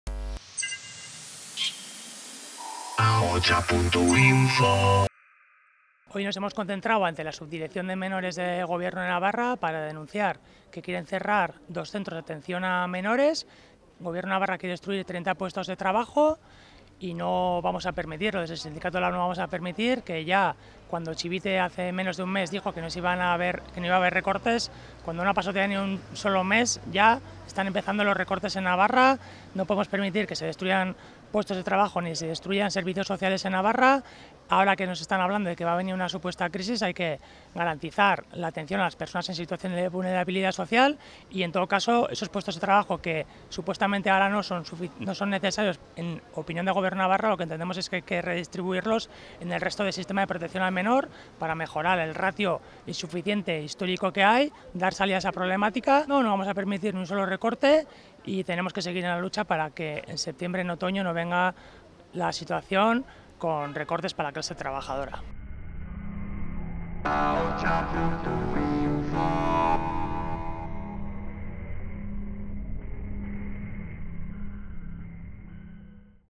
LAB denuncia recortes en la atención a menores en Nafarroa con el cierre de 2 centros
El sindicato LAB se ha concentrado hoy ante la Subdirección de Atención a la Infancia del Gobierno de Nafarroa para denunciar las intenciones del Ejecutivo de María Chivite de cerrar dos centros de atención a menores y la destrucción de 30 puestos de trabajo.